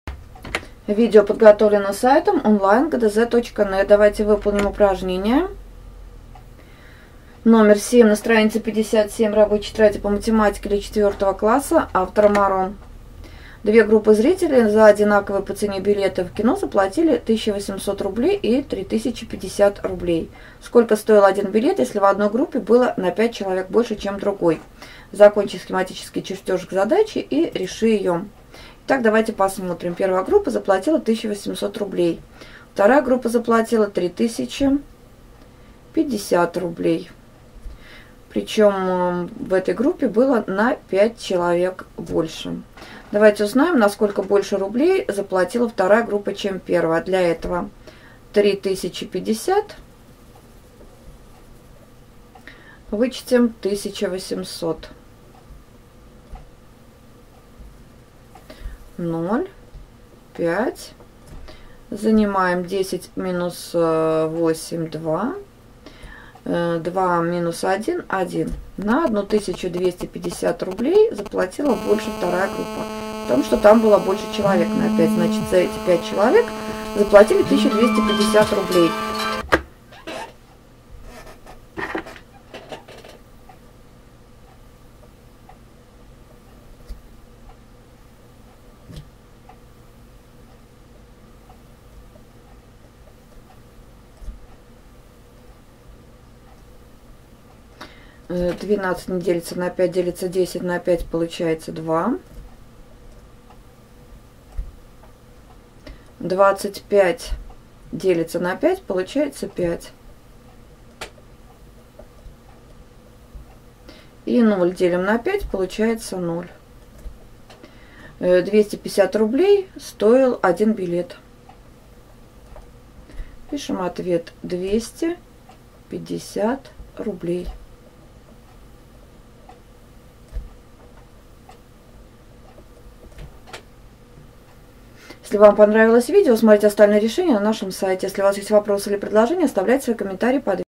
Задание №6 (2) Деление на двузначное…- ГДЗ по Математике Рабочая тетрадь 4 класс (Моро) 2 часть
Чтобы выполнить задание №6(2) на странице 56 для этого необходимо уметь заканчивать схематические чертежи к задачам, решать составные задачи в два действия, выполнять вычитание многозначных чисел, деление четырехзначных чисел на однозначные числа. Вы можете выполнить это задание за 2 - 3 минуты, если посмотрите наше видео.